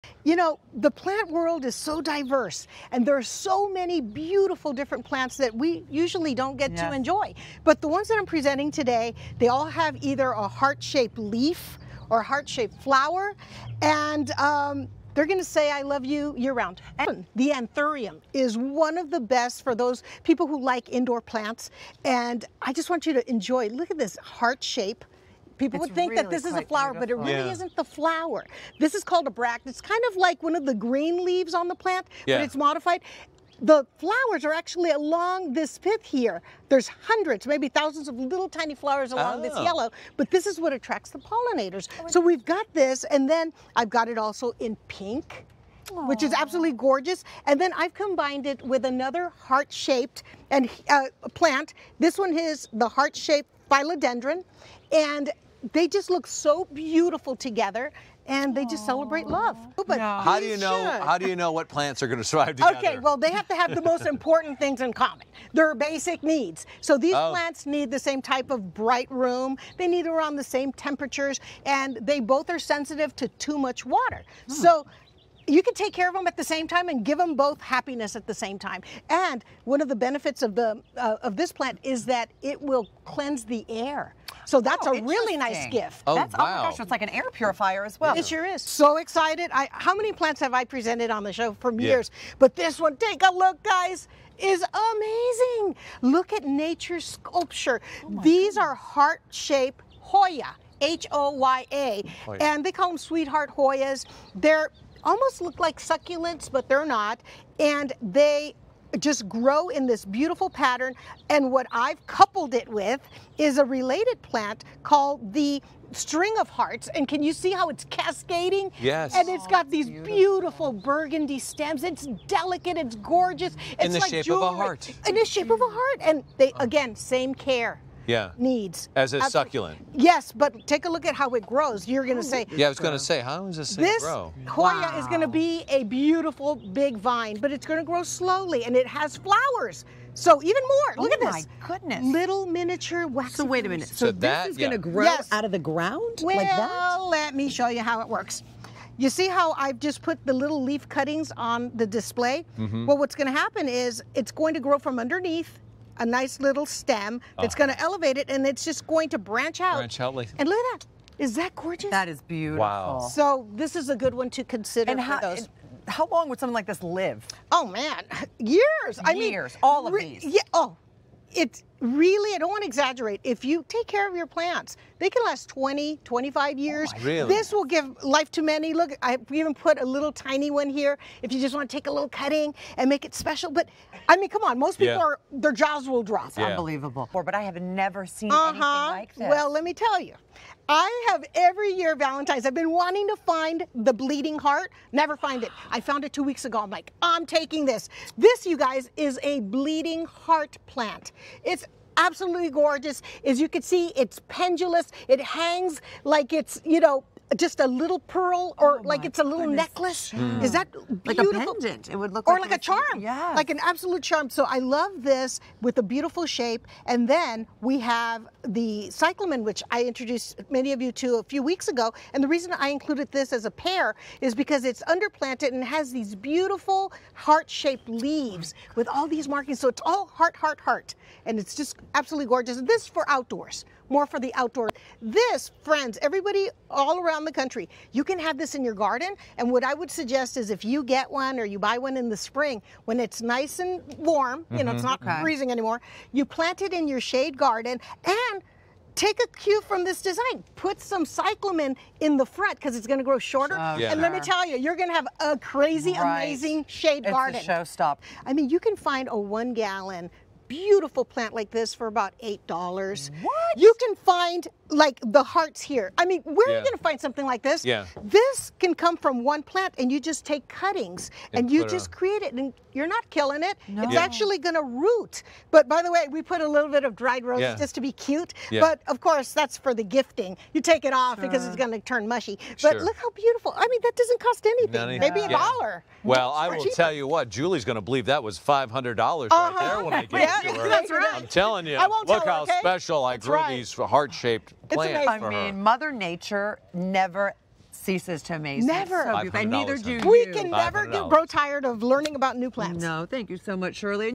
Plants with Heart Shaped Leaves and Flowers
Plants with heart shaped leaves and flowers are nature's artwork and are a great alternative to cut flowers to say "I Love You!" Anthurium, Heart shaped Hoya, Chain of Hearts, Bleeding Heart plants and more.

Shirley B presents great indoor and outdoor plant combinations that are heart inspired and look great together.

EdenMakersBlog.com
Facebook.com/EdenMakers
Instagram.com/ShirleyBovshow